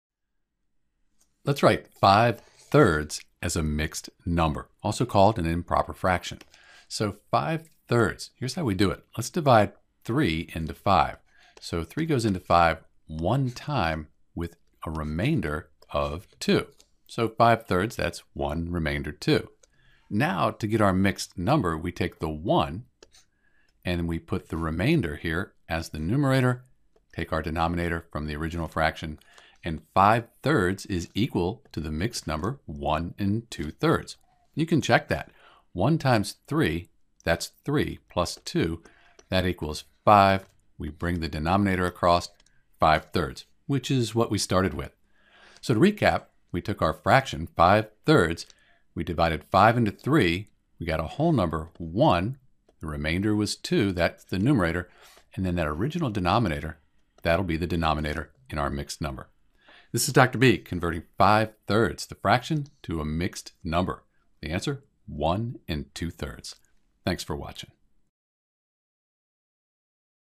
Write 5/3 as a Mixed Number
In order to write 5/3 as a mixed number in simplest form (sometimes people will say "as a whole number" or "mixed fraction") we can follow a few steps.

Note that 5/3 is already simplified (in lowest terms). So we can't reduce or simplify 5/3 any further.  But we can write is as a mixed number.

First we can divide the numerator (5) by the denominator (3).  3 goes into 5 a total of 1 times with a remainder of 2.

Second, we take the remainder and put it over the original denominator.  This gives us 2/3.

For the mixed number we have the whole number 1 and the fraction (from the remainder) of 2/3.

So 5/3 written as a mixed number is 1 2/3 (one and two-thirds).

To check your work you can now multiply the whole number in mixed fraction by the denominator (1 x 3) and add this to the numerator (the 2 in the 2/3) you will get 4/3.  This is a good way to check that you have the right mixed fraction.